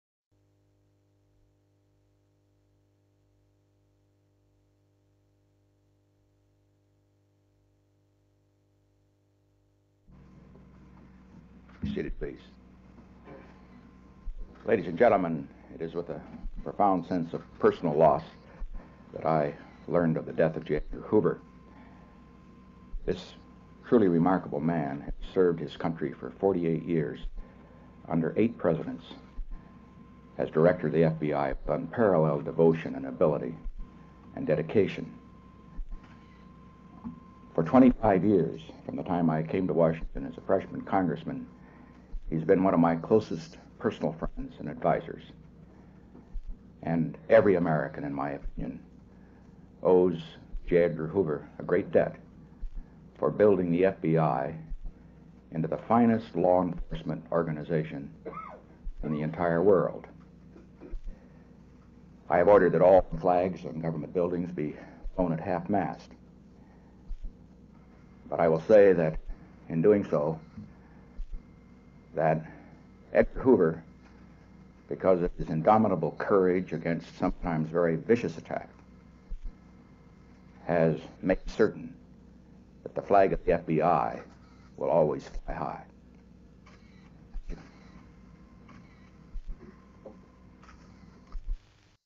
May 2, 1972 - President Richard Nixon's Remarks on the Death of FBI Director J. Edgar Hoover
President Richard Nixon spoke in the Briefing Room at the White House. He spoke without referring to notes.
Ladies and gentlemen:

It is with a profound sense of personal loss that I learned of the death of J. Edgar Hoover. This truly remarkable man has served his country for 48 years under eight Presidents as Director of the FBI with unparalleled devotion and ability and dedication.

For 25 years, from the time I came to Washington as a freshman Congressman, he has been one of my closest personal friends and advisers. Every American, in my opinion, owes J. Edgar Hoover a great debt for building the FBI into the finest law enforcement organization in the entire world.

I have ordered that all the flags on government buildings be flown at half-mast. But I will say that in doing so, Edgar Hoover, because of his indomitable courage against sometimes very vicious attack, has made certain that the flag of the FBI will always fly high.
Mr. Hoover, 77, died at his home in Washington, D.C.